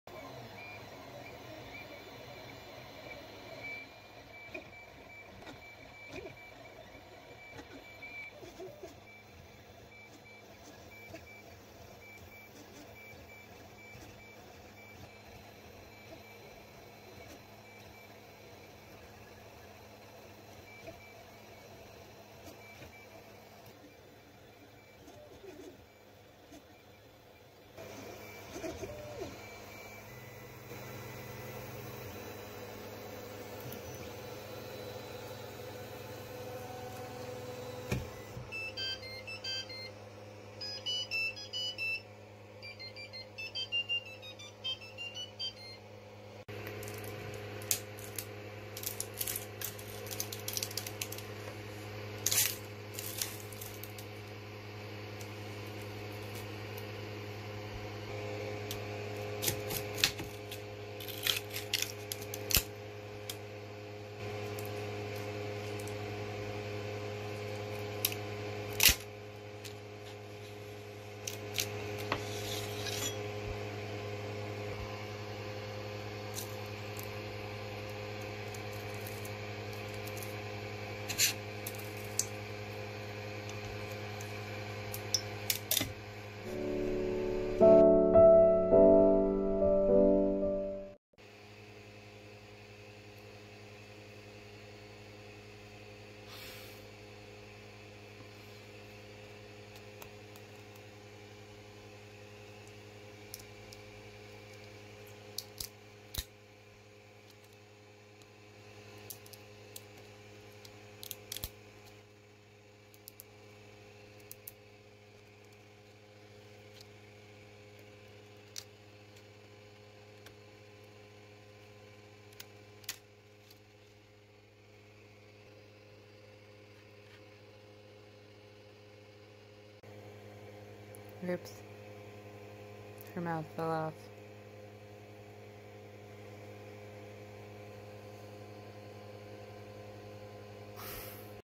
Free 3D printable Vampira Stencil by nicoledobbins on Thingiverse
This is my first try printing this model, and I ripped half her face off. 🤷‍♀️ HELLHOUND!
See this stencil get rescued and put to use in my upcoming videos

I made this stencil for the denim vest I'm painting for when my husband and I go see The Original Misfits in September at White River Amphitheatre.  I look forward to seeing everyone at the show, I'll be up on the Lawn and hoping my painting supplies don't get confiscated, because I'm planning to stencil people.

WHERE TO GET PRODUCTS USED IN THIS VIDEO
Open Source 3D Printer in use: creality CR-10
Purple PLA 3D printing filament: Stronghero3D

How I glued Vampira's mouth back on:
E6000 Allure Gloss Dimensional Adhesive Paint